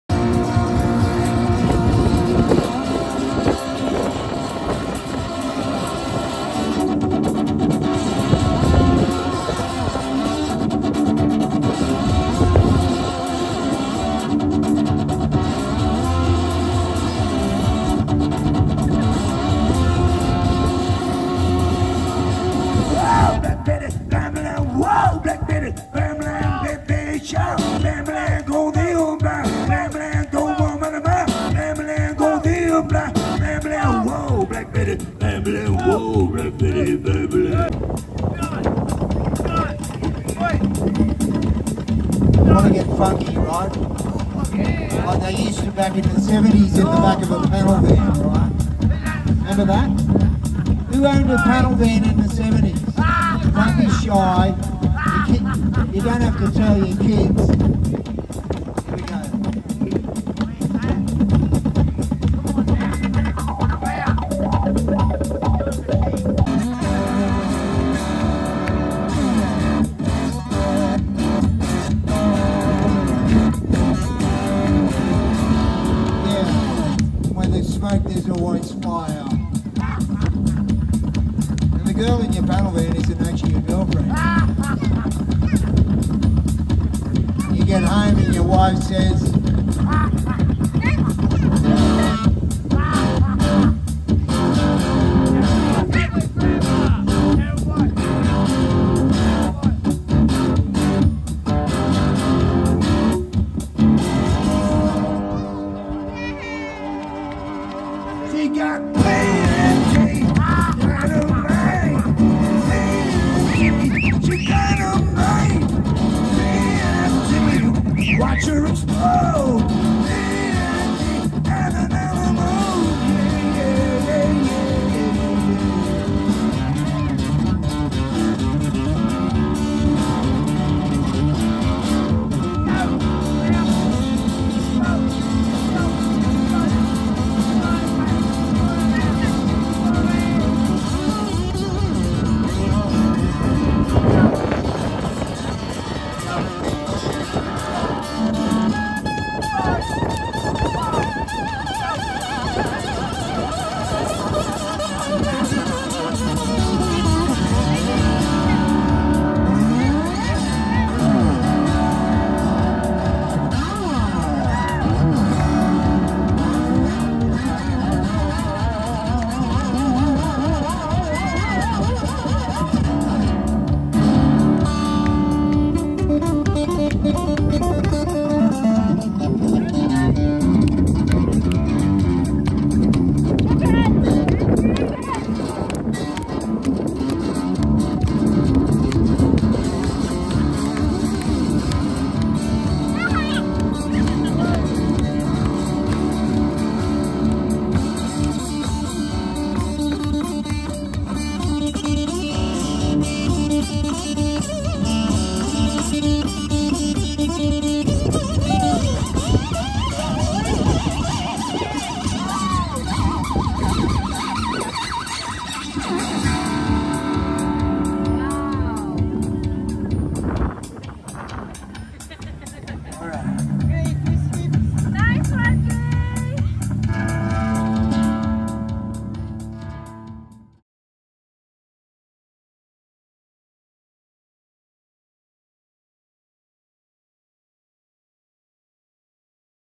Special guest loving it - Snapper, rainbow bay and Green mount SURF
Surf at Rainbow Bay and Green mount was pretty average as usual - With long fast waves hollow crowded as usual - special guest had the best time loving the tunes at tweed heads - Check it out - You will love it

Follow us on:
Facebook: Life in The Wave